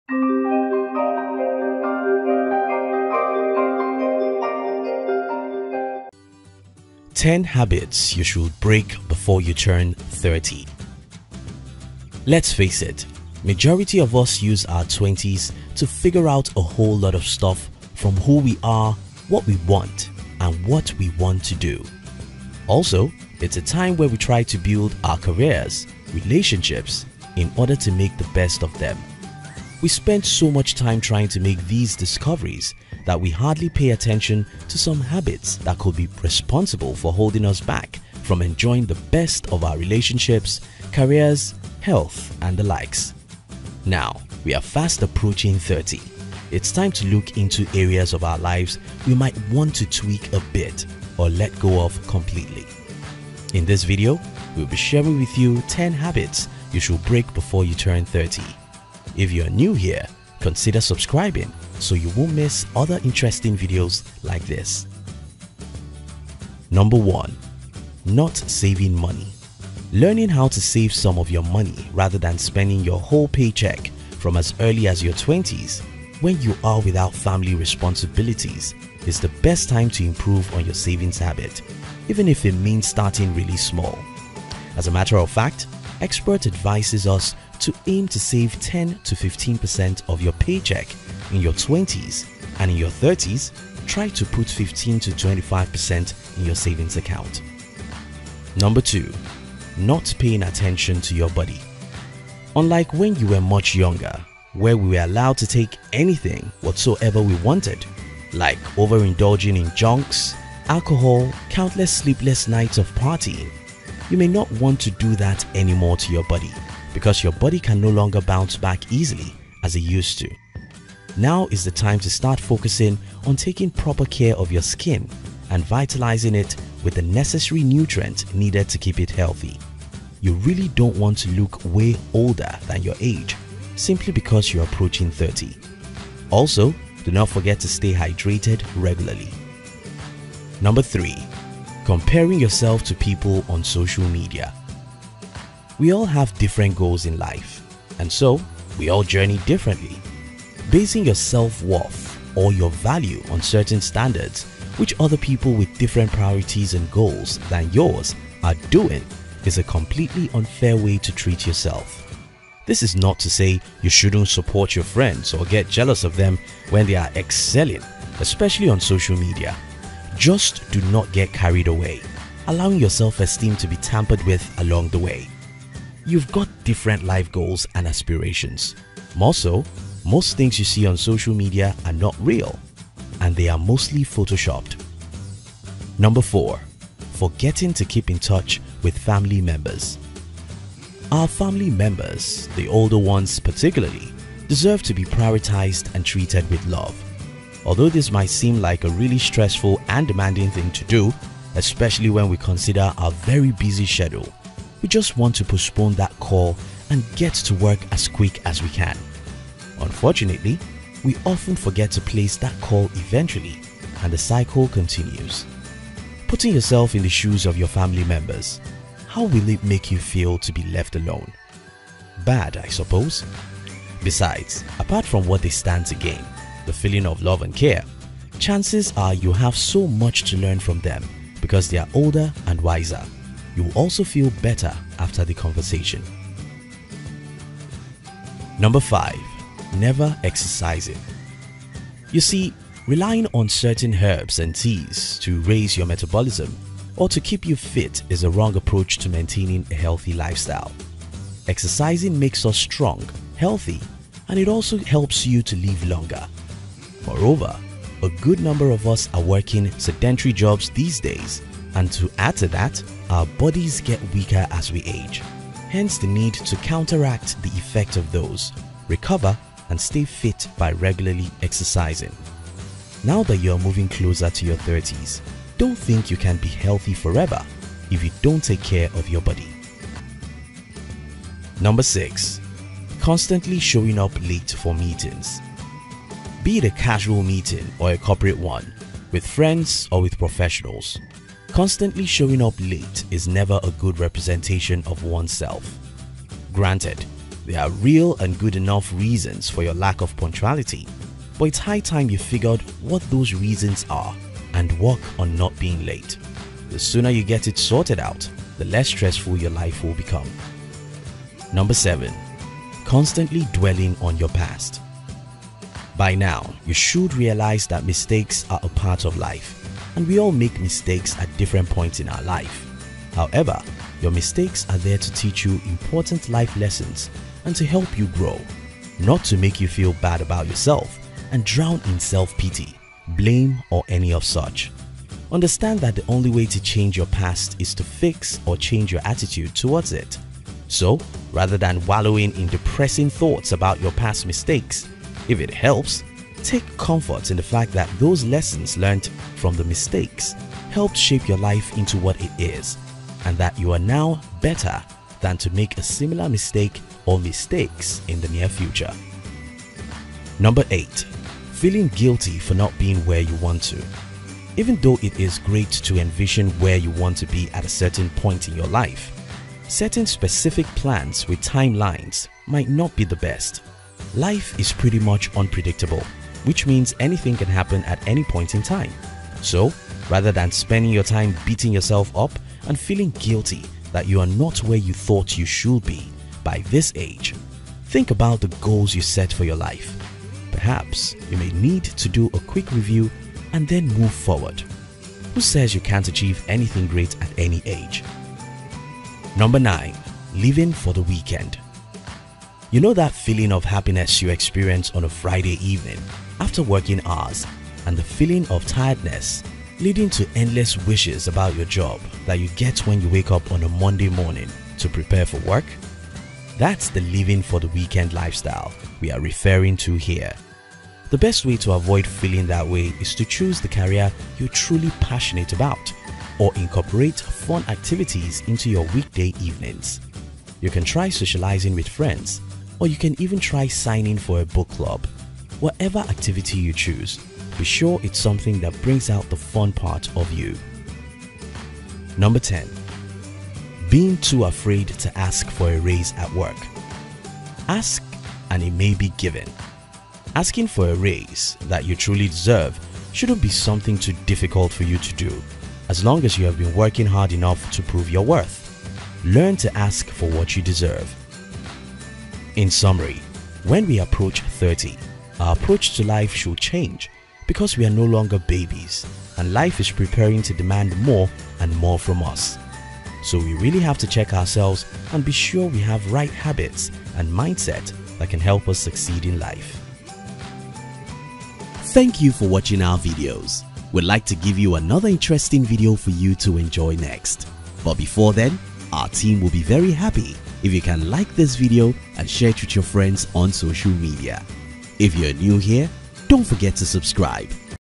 10 Habits You Should STOP Before 30
In this video, We're going to share with you the 10 Bad Habits You Should Stop before 30.
1. Not saving Money
2. Not paying attention to your body
4. Forgetting to keep in touch with family members
5. Not exercising
6. Constantly showing up late on meetings
7. Dwelling on your past
8. Feeling guilty for not being where you want to
9. Enjoying the weekend too much
10. Being afraid to ask for what you deserve

EXCLUSIVE:
The Rich Dad Summit | Robert Kiyosaki - How To Earn a 7 FIGURE Income